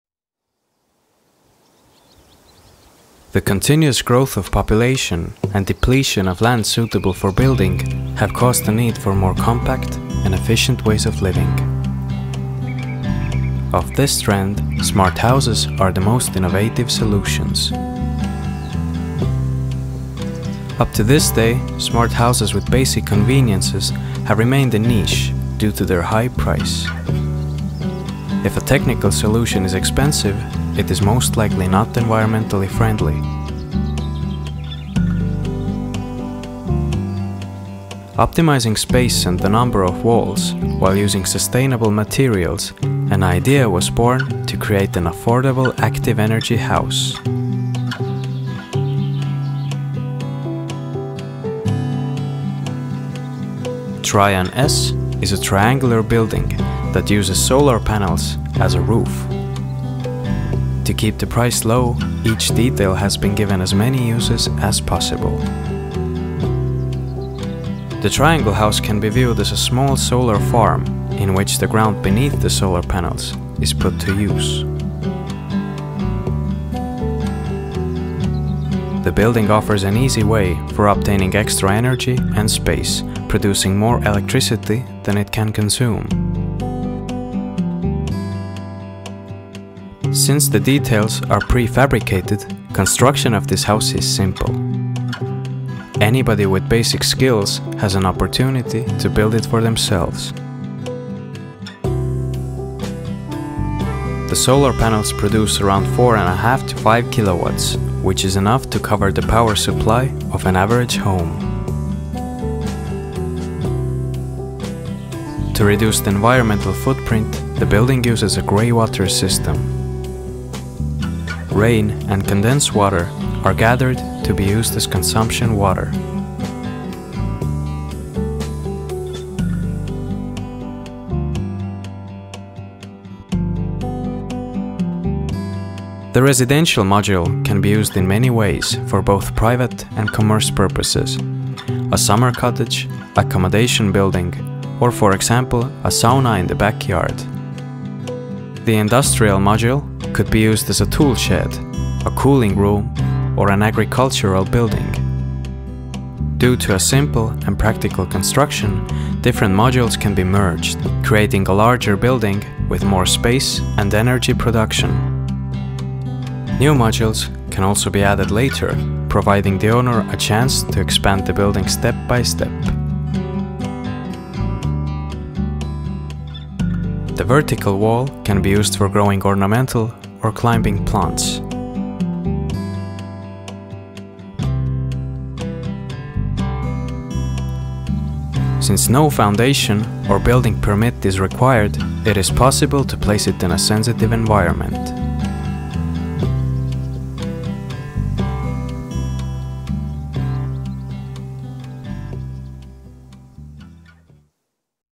Modular house "Trian-S"
Trian-S is a self-sufficient modular house that produces energy.
Filmed in Soonlepa, Hiiumaa, Estonia.

Video and graphics by Sander Koit.